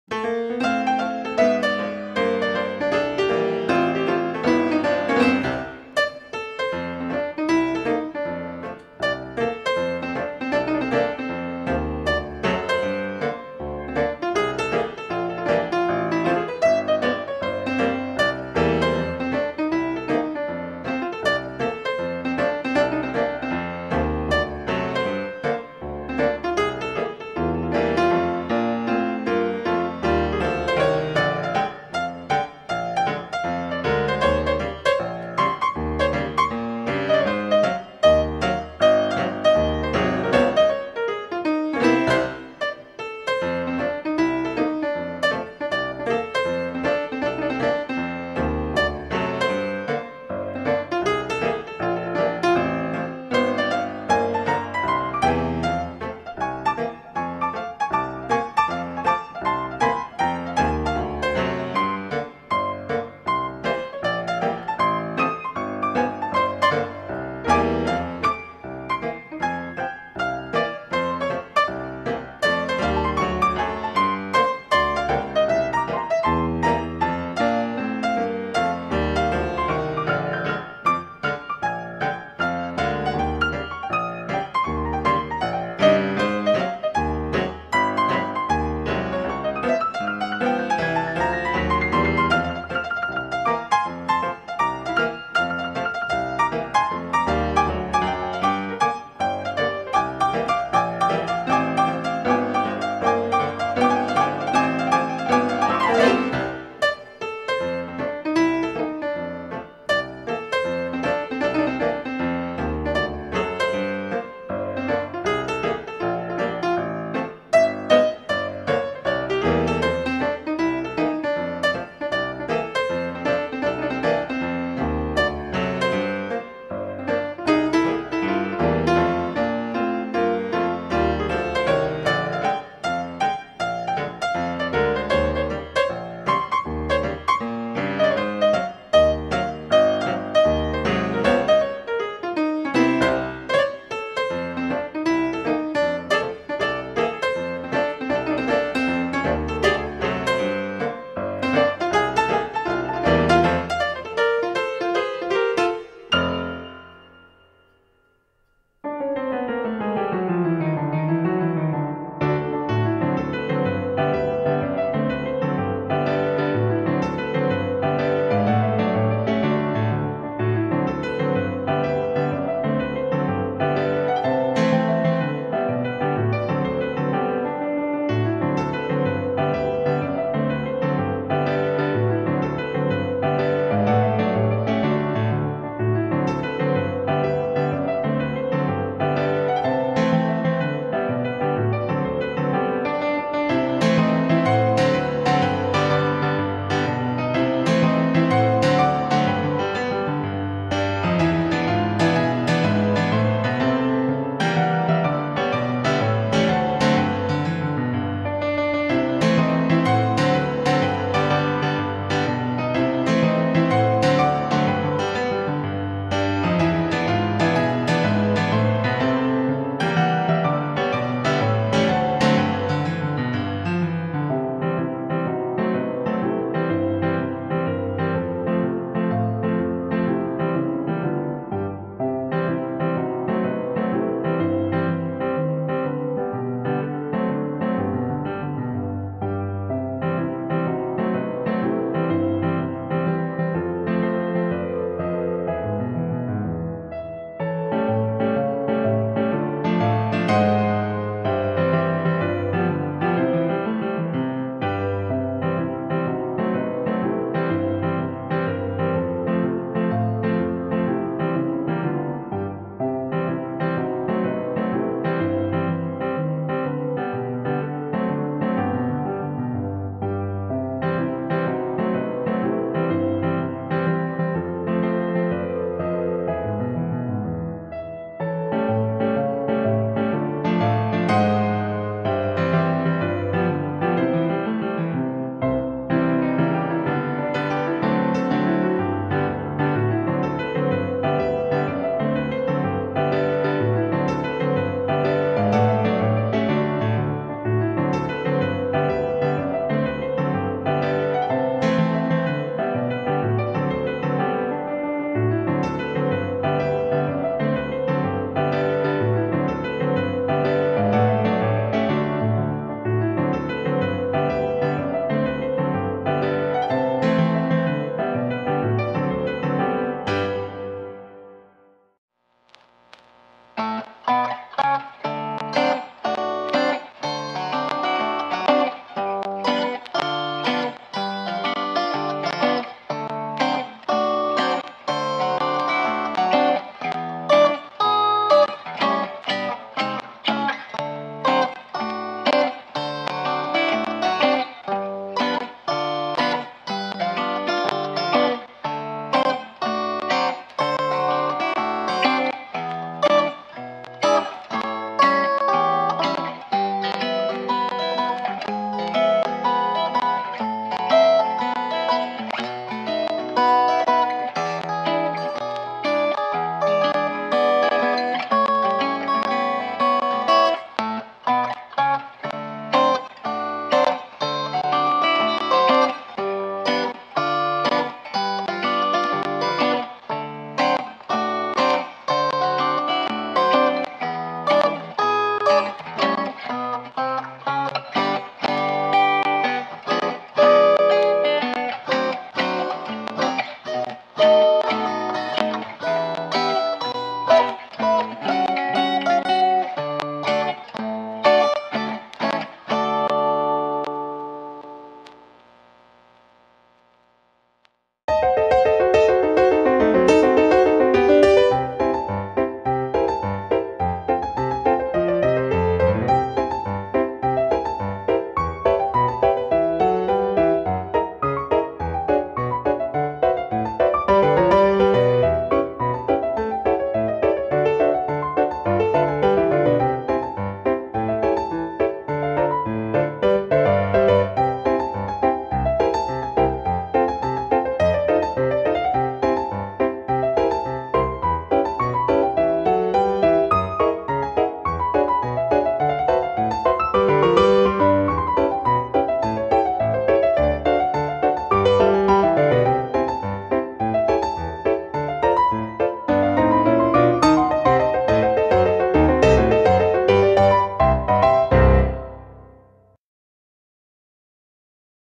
More Amazing Vaudeville Archival Footage (2/2)
More archival footage of vaudeville acts from a simpler time, with light ragtime music.